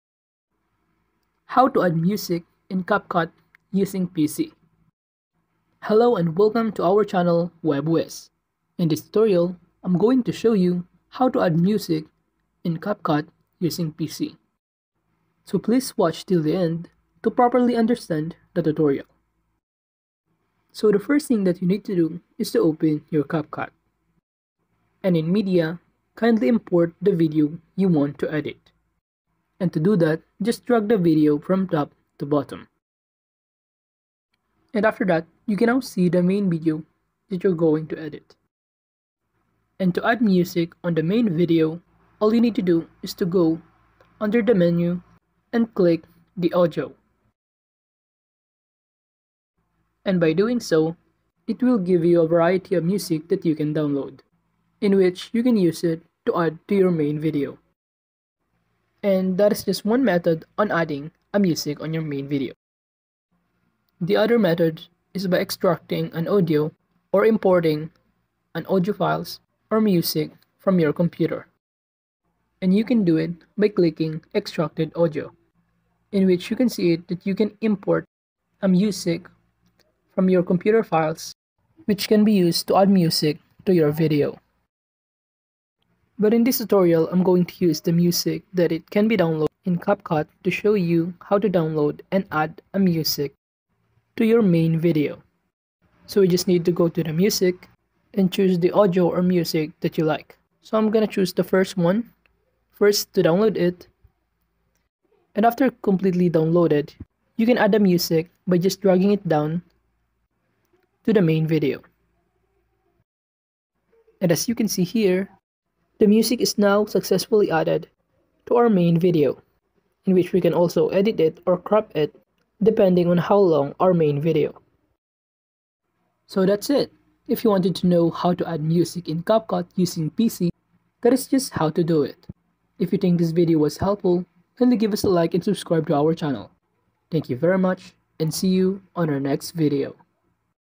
How to Add Music in Capcut PC 2024
How to Add Music in Capcut PC from files or device